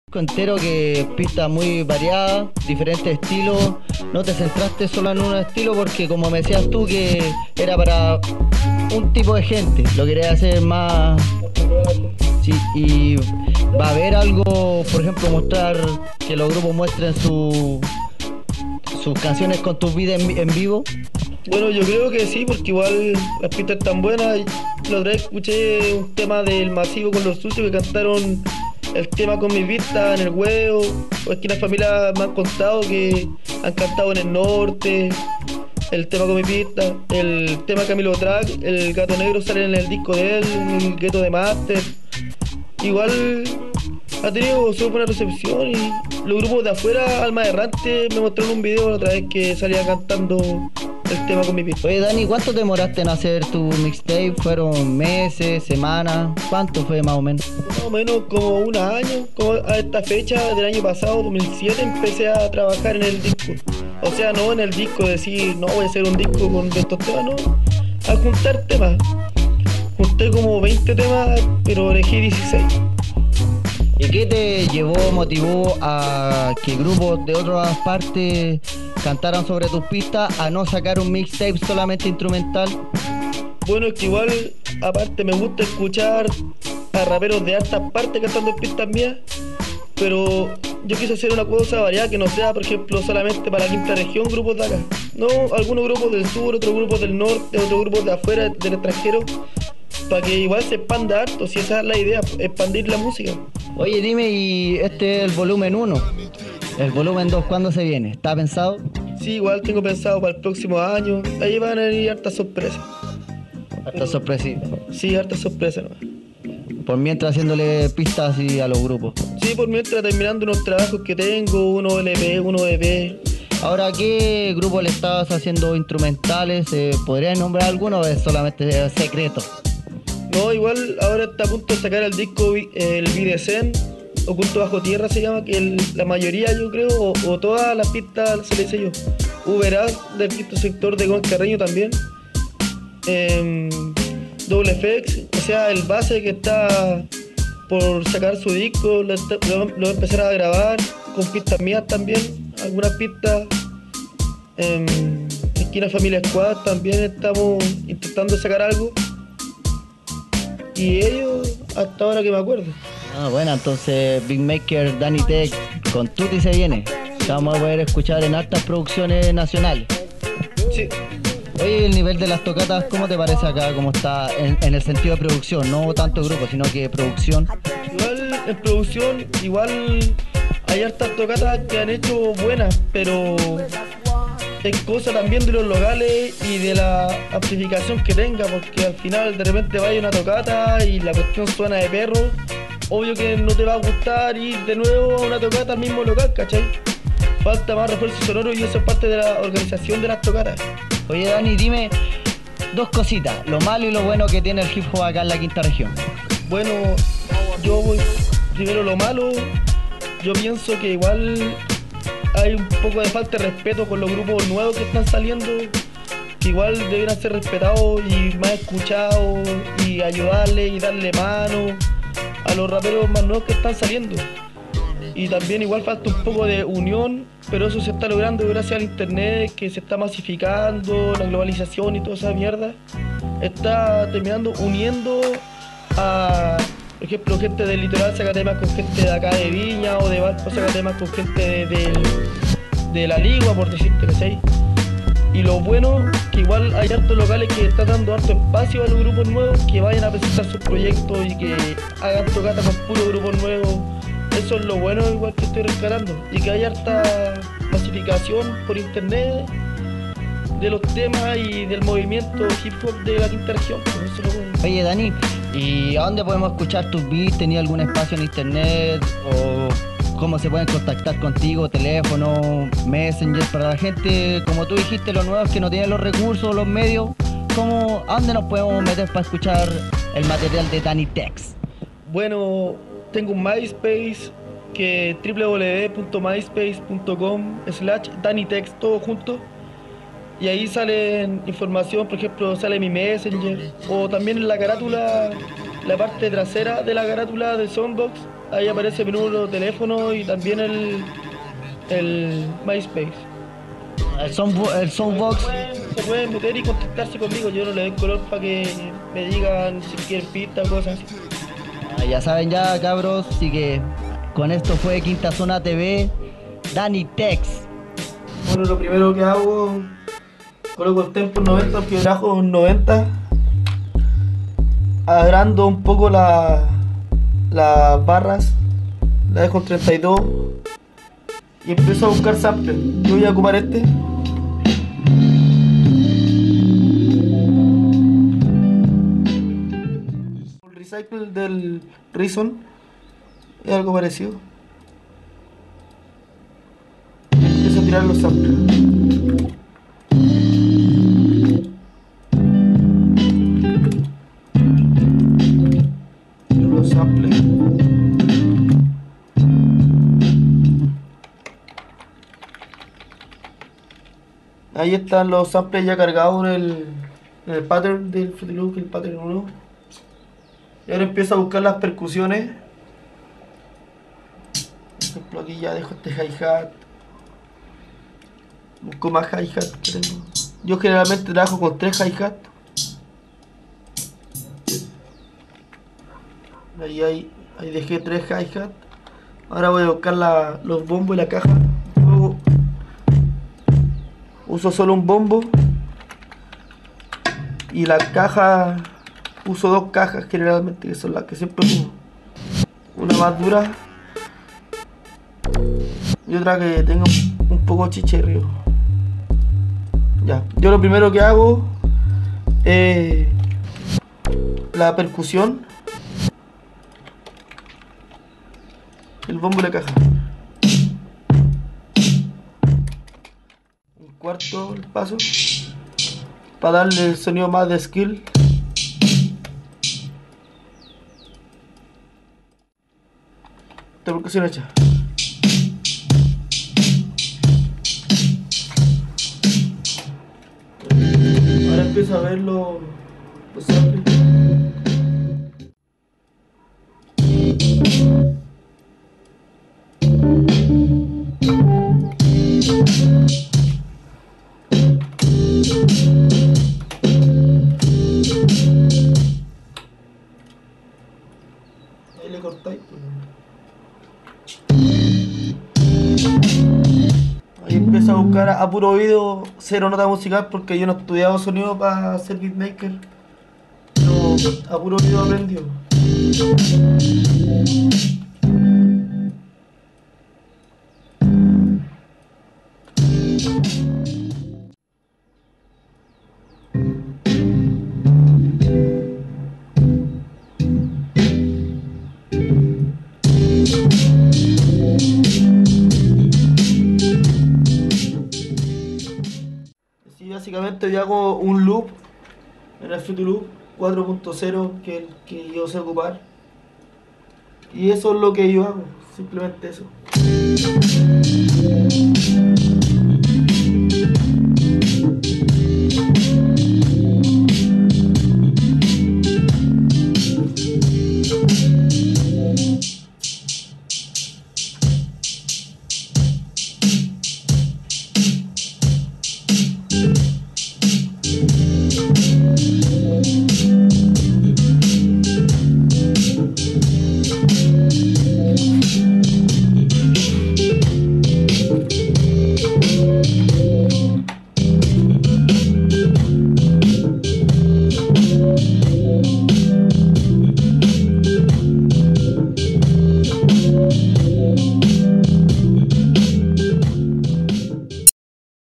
entrevista a DANNY TEKS 2do capitulo KINTAZONA TV
segunda parte entrevista beatmaker danny teks